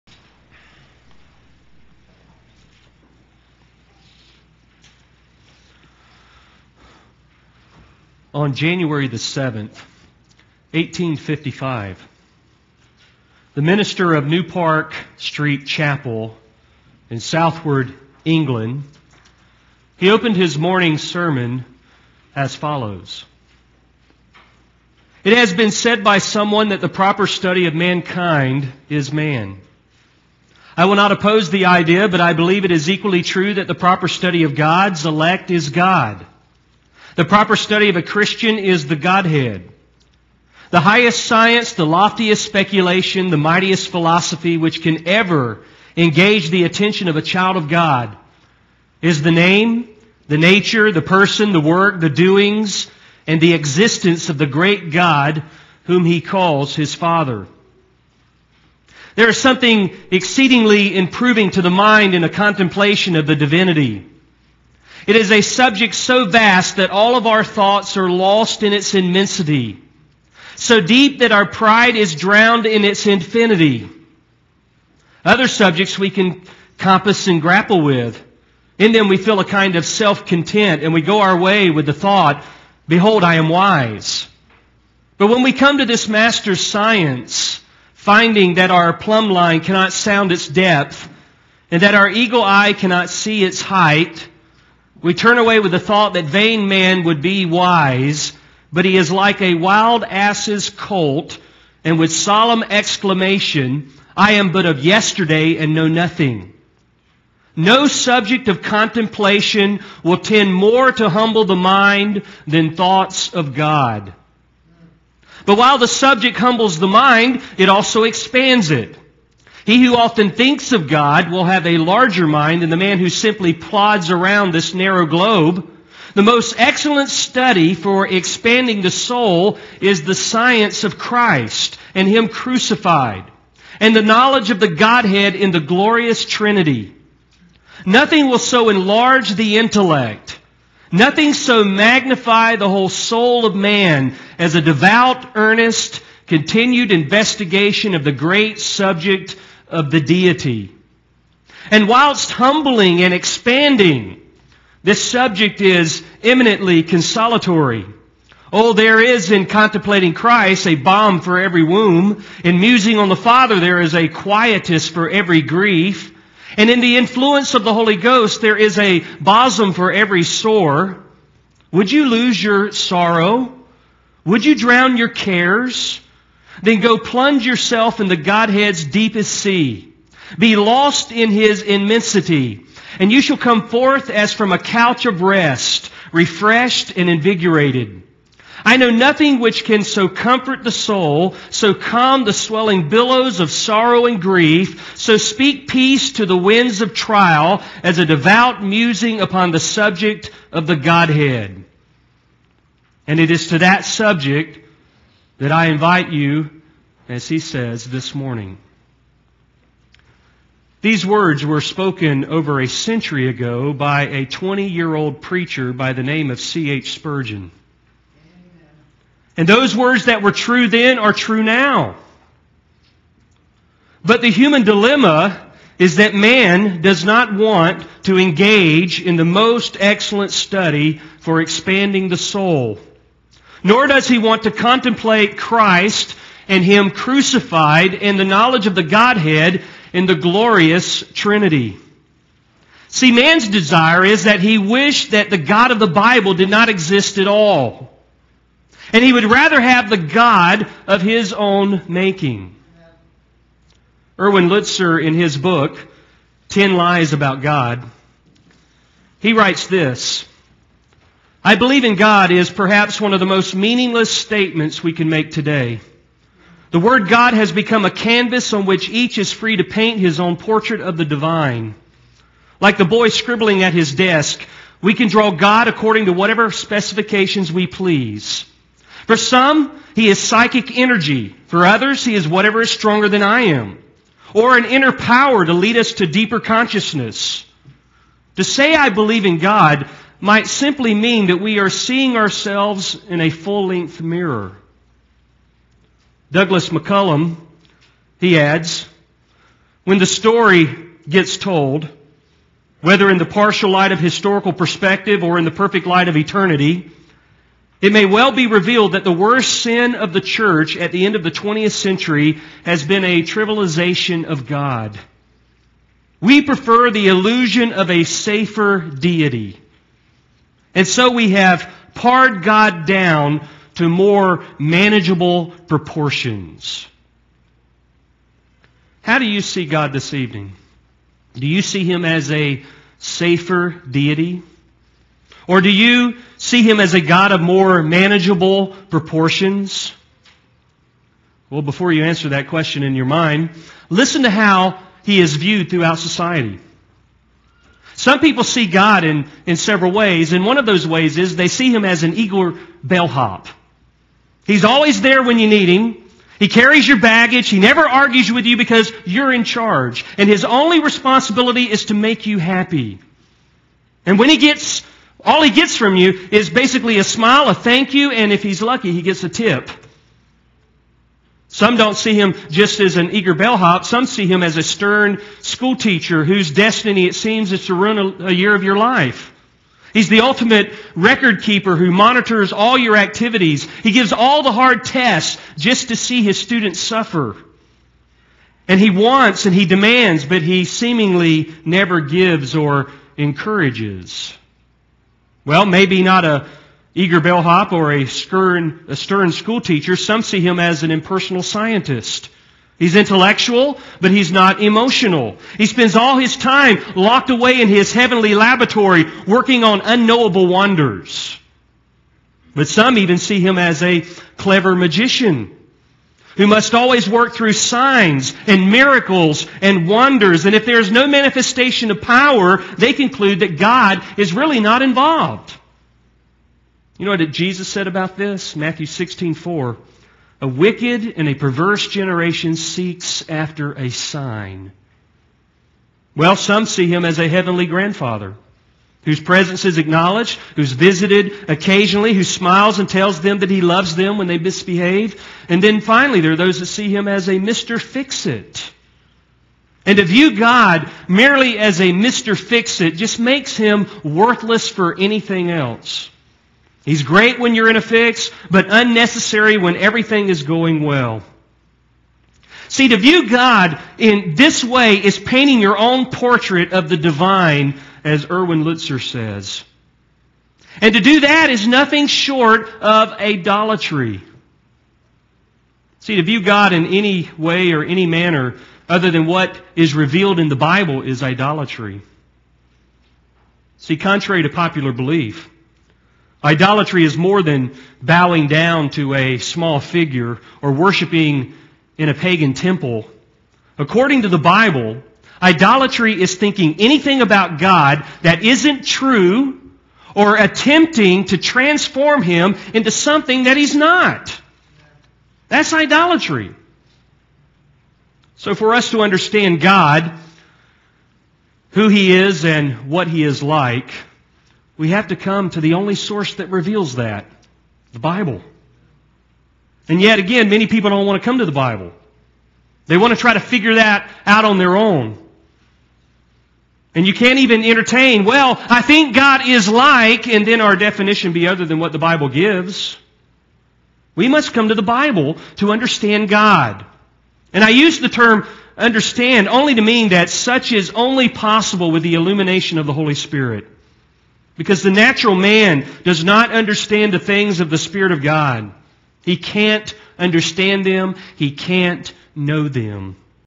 Who is God?
In this video, Pastor Steve introduces us to the God of the Bible.